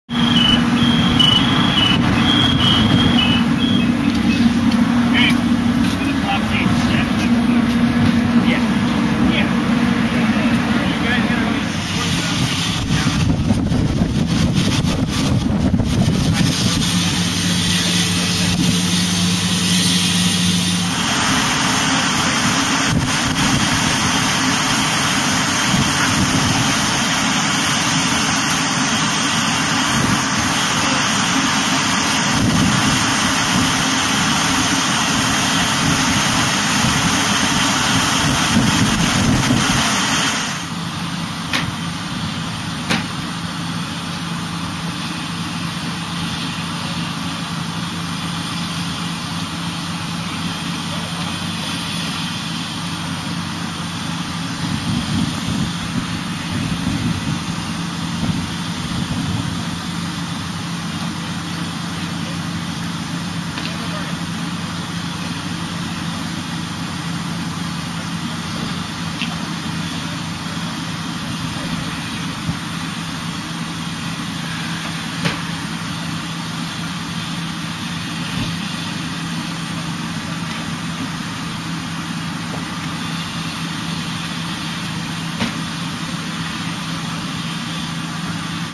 House Fire on Kingsbury Grade
A shake roof caught fire on March 20, 2008, in an Upper Kingsbury neighborhood, causing damage to the home.